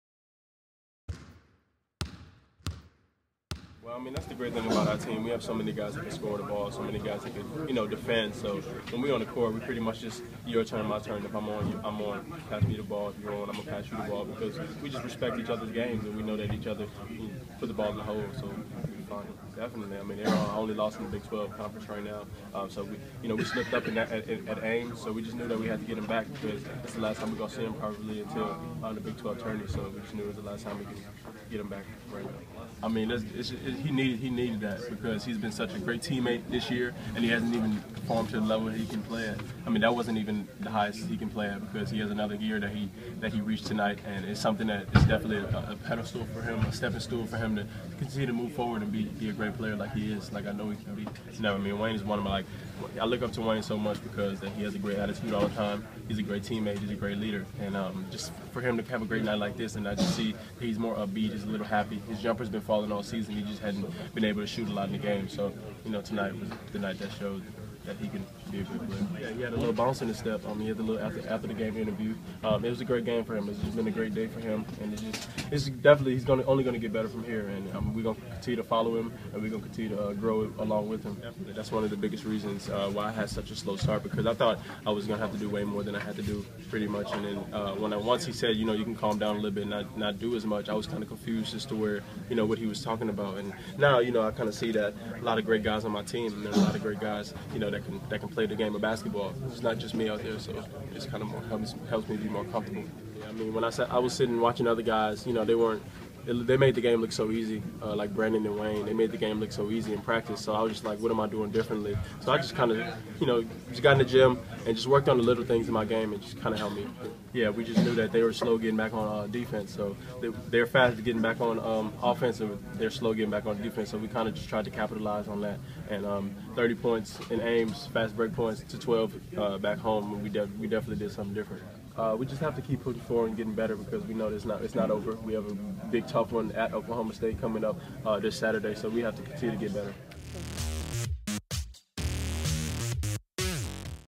Oubre on KU owing ISU and importance of Selden
Following the Kansas basketball team's Big Monday home win over Iowa State, freshman Kelly Oubre Jr. discusses KU owing ISU, having lost in Ames. Plus, the freshman talks about the importance of Wayne Selden Jr. to KU's success.